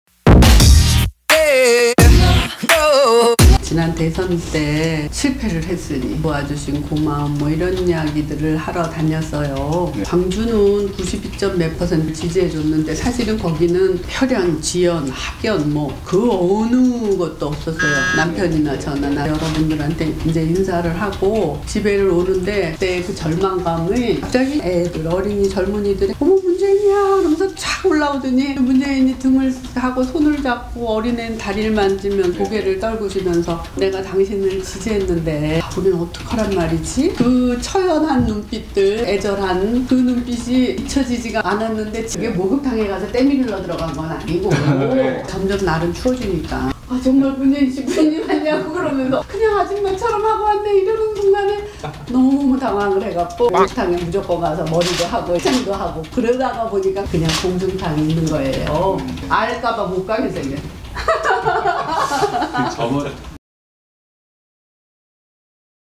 [Video C] 문재인 부인, 김정숙과의 대담
2012년 대선 이후 광주에서 올라오는 길에는 미안함의 눈물이